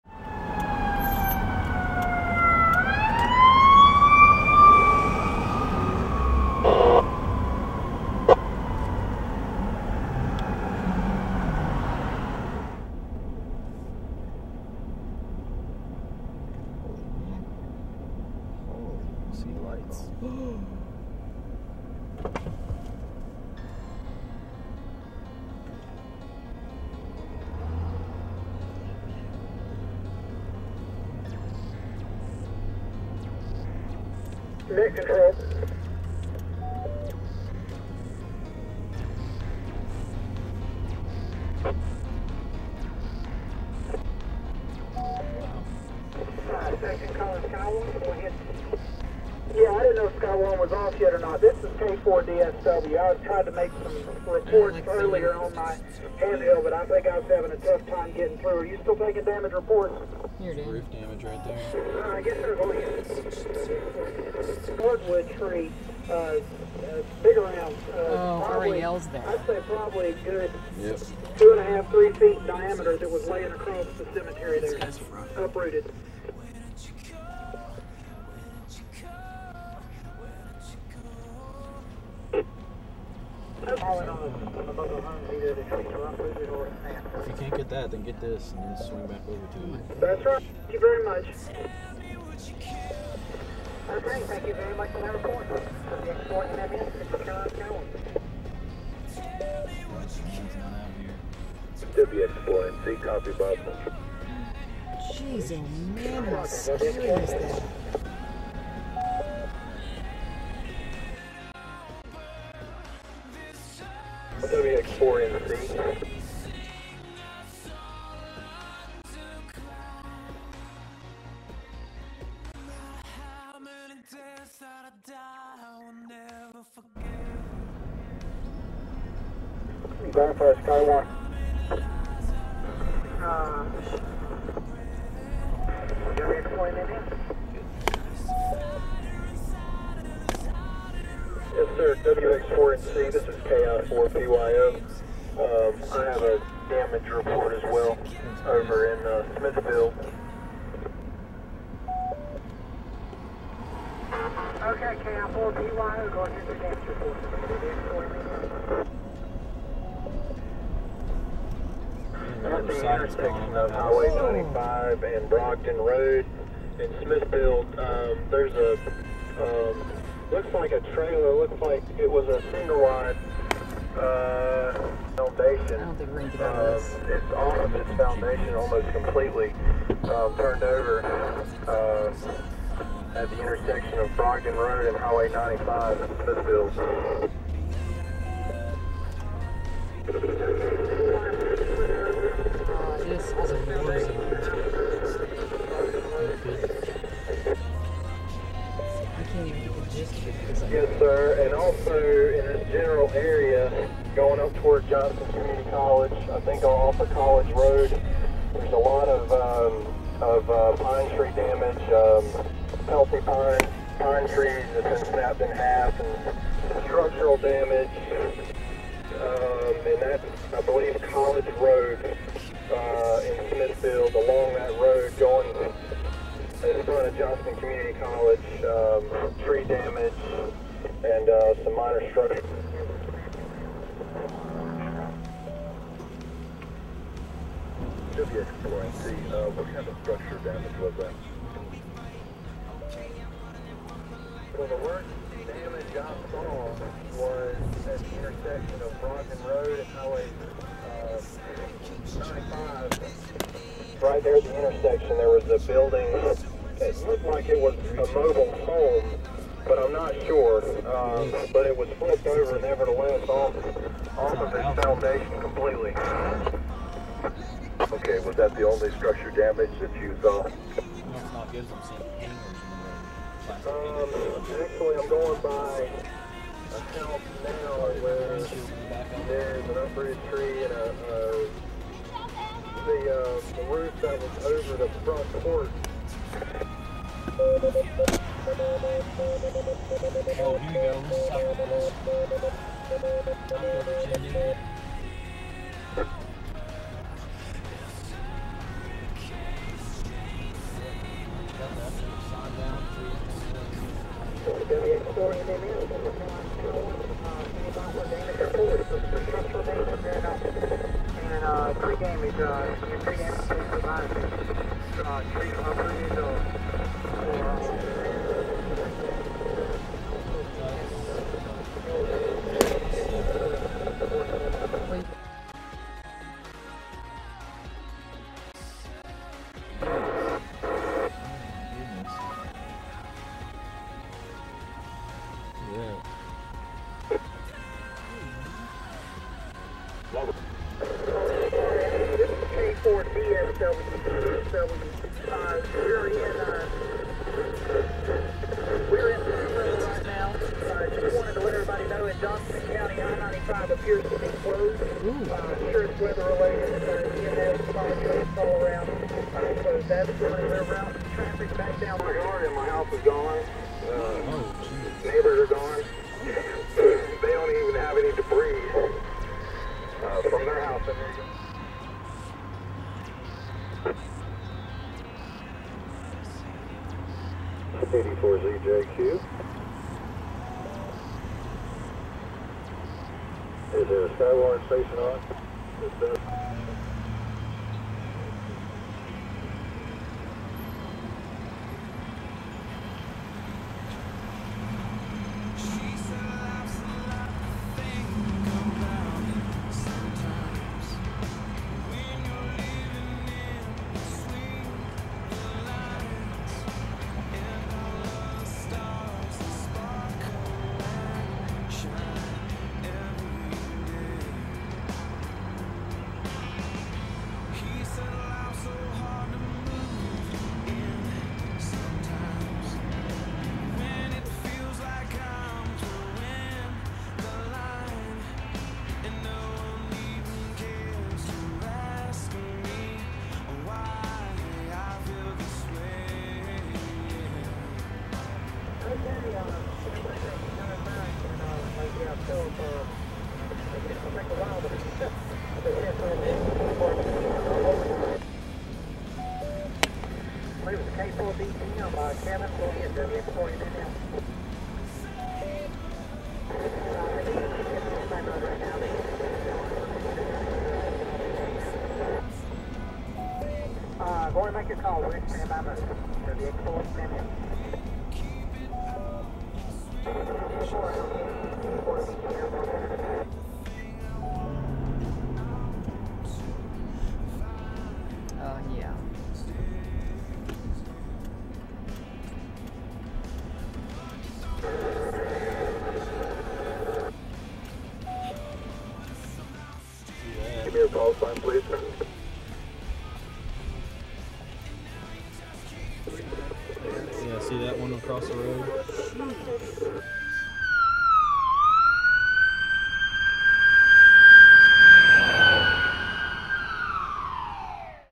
SkyWarn Active as Tornados Touchdown - Raleigh NC, April 2011
April 16, 2011 Aftermath Damage. This is only a glimpse of what the city saw. There were a reported 7 touchdowns in Wake County, and 74,000+ power outages. There was everything from roof damage, overturned cars, and houses being completely demolished or missing from their foundations. Central Carolina Skywarn was active for a majority of the day, and long after I went to sleep.

---Count as of 4/18/11 10AM - 22 Dead - R.I.P. ---

*It was pointed out that in my rush to edit and get this video up I mis-spelled April in the title... Sorry guys!*